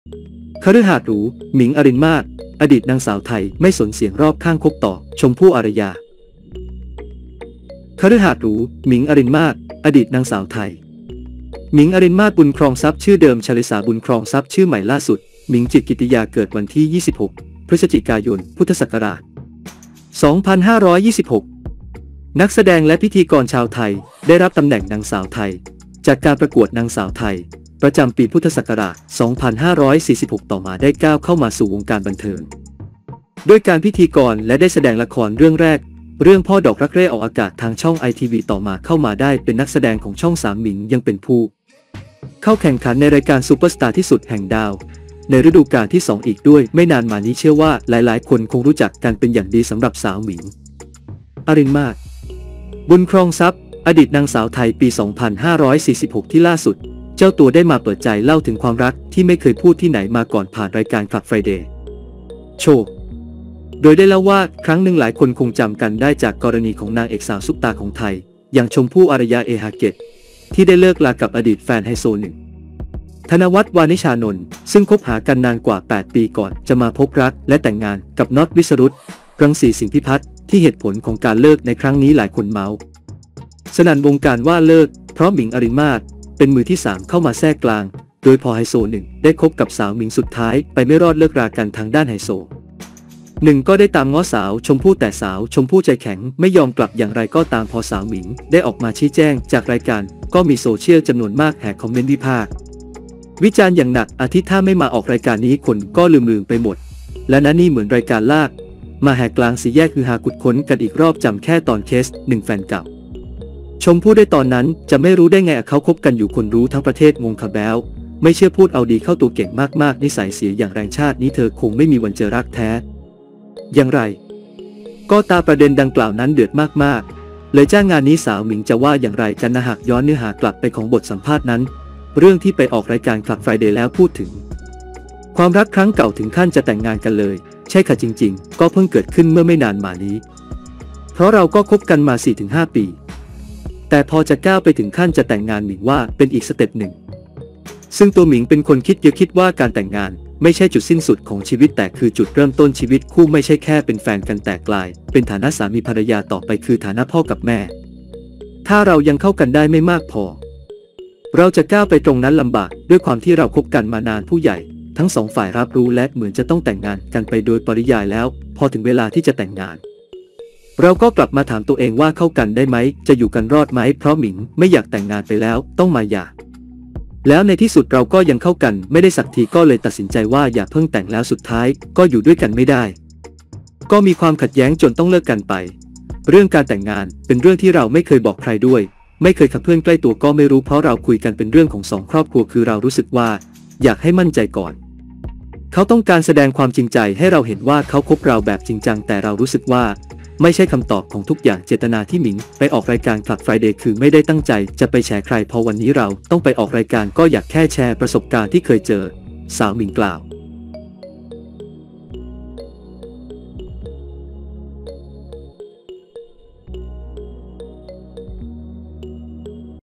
คฤหาสน์หรู ‘หมิง อรินทร์มาศ’ อดีตนางสาวไทย ไม่สนเสียงรอบข้างคบต่อ ‘ชมพู่ อารยา’
ทุ่มกว่า 100 ล้าน หลังเข้าแทรกกลางนางเอกดัง
คฤหาสน์หรู 'หมิง อรินทร์มาศ' อดีตนางสาวไทย
ไม่สนเสียงรอบข้างคบต่อ 'ชมพู่ อารยา' แม้โดนล่มวิวาห์ซ้ำซ้อน